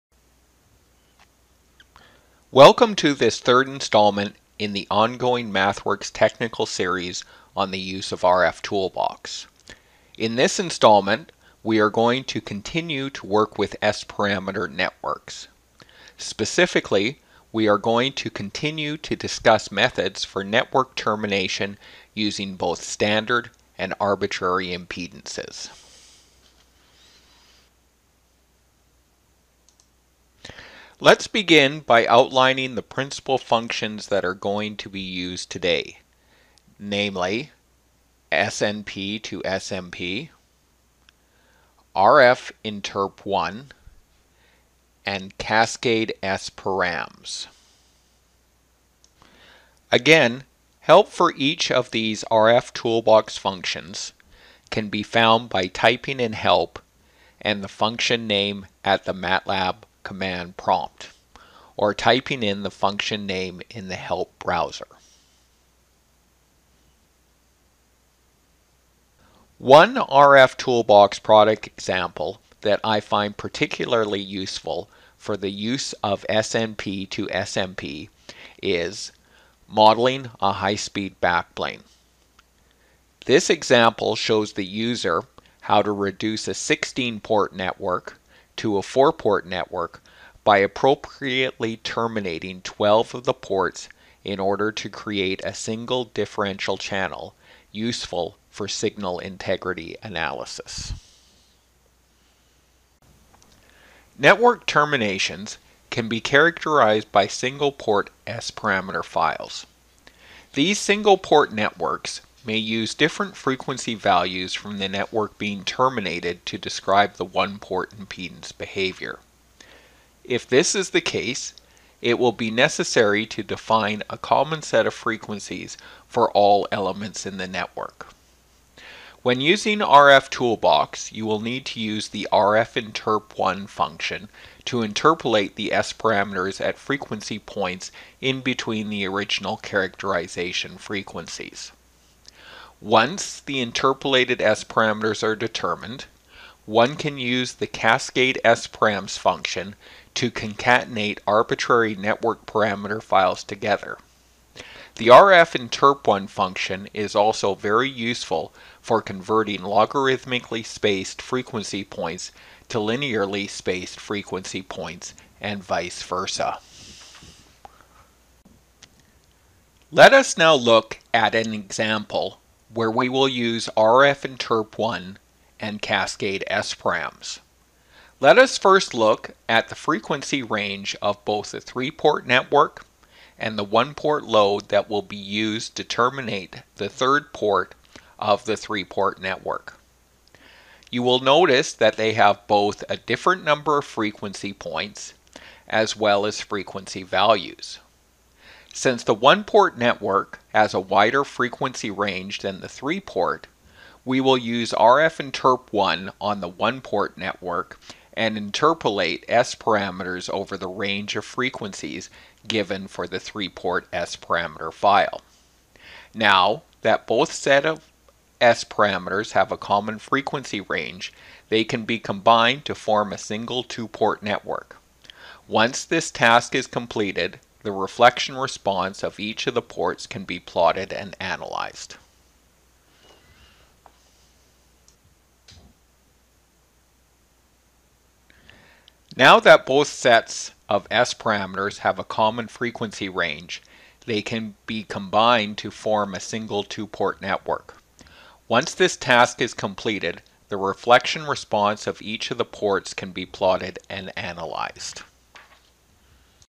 Termination of S-Parameter Networks | Getting Started with S-Parameters, Part 3
Learn how to interpolate S-parameter data over different frequency ranges. See how to apply arbitrary port terminations and verify how different impedance conditions impact the network performance.